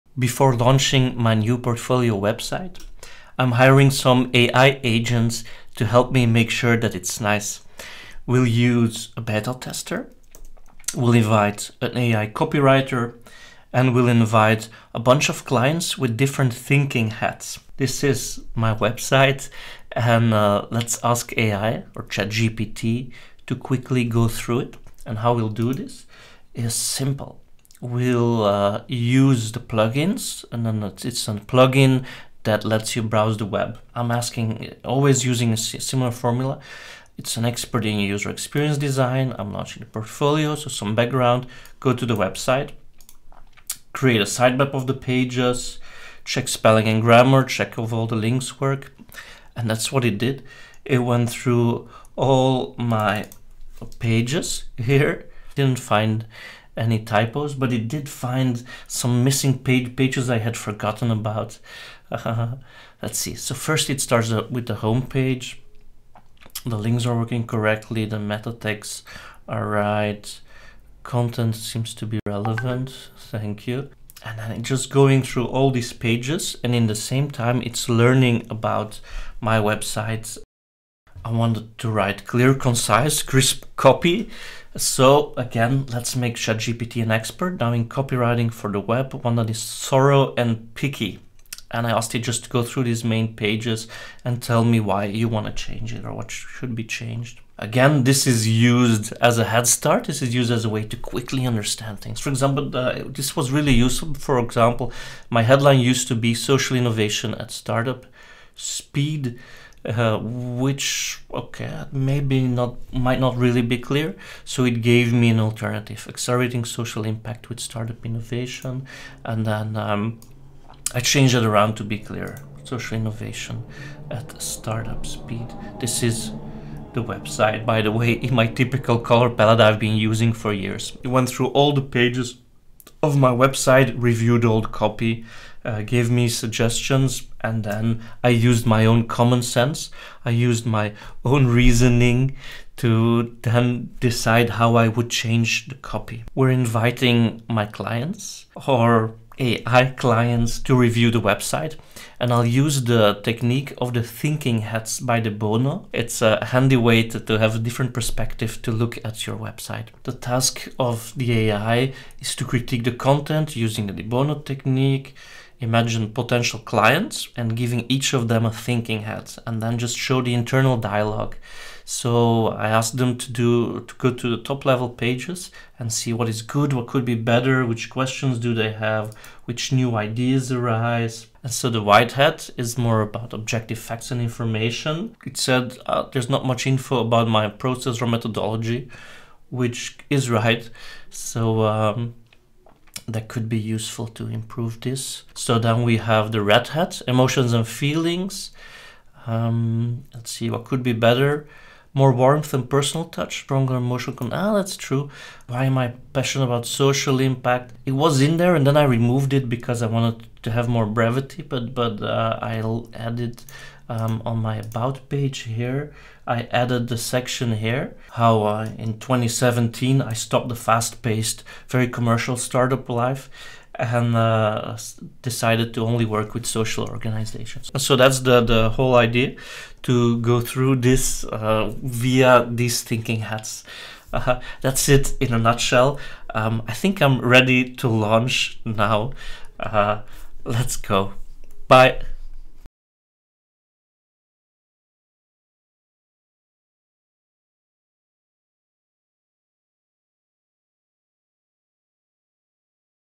Review your website with chatGPT
ChatGPT can read your website.
So I asked it to become a betatester, copywriter and 6 clients.
Each client having a different thinking hat 📖 💗 🌚 🌻 🌿 🌐

It helped me double-check my updated service design portfolio:
- Uncover outdated and broken pages.
- Improve the copywriting.
- Realise I need more impact numbers.

Is there anything that the Ai missed?
Real human feedback is still valuable and very welcome :)